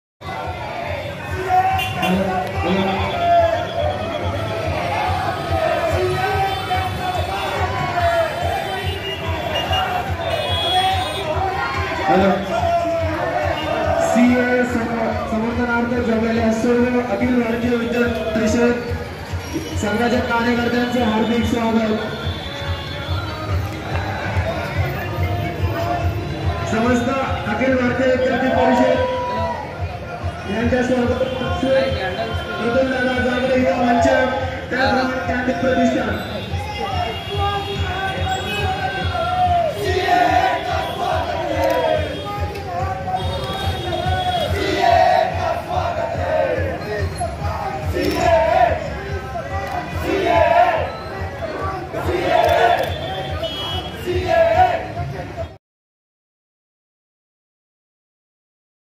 पुण्यात सीएएच्या समर्थनार्थ ३०० फूट लांब "तिरंगा पदयात्रा "
पुणे : देशात एकीकडे केंद्र सरकारच्या सुधारित नागरिकत्व कायद्याच्या विरोधात आंदोलने आणि निदर्शने होत आहेत. तर दुसरीकडे कायद्याच्या समर्थनार्थ रॅली निघत आहेत. दरम्यान सुधारित नागरिकत्व कायद्याला आज अशाच पद्धतीने पुण्यात  पाठिंबा देण्यात आले.

केंद्र सरकारने लागू केलेल्या सीएए कायद्याच्या स्वागत आणि समर्थनार्थ पुणे महानगर अखिल भारतीय विद्यार्थी परिषदेने ३०० फुट तिरंगा पदयात्रा काढली. ही पदयात्रा मॉर्डन महाविद्यालय गणेश खिंड ते शिवाजीनगर मॉर्डन महाविद्यालय पर्यंत काढण्यात आली. दरम्यान, या पदयात्रेला विद्यार्थ्यांचा उत्स्फूर्त प्रतिसाद मिळाला.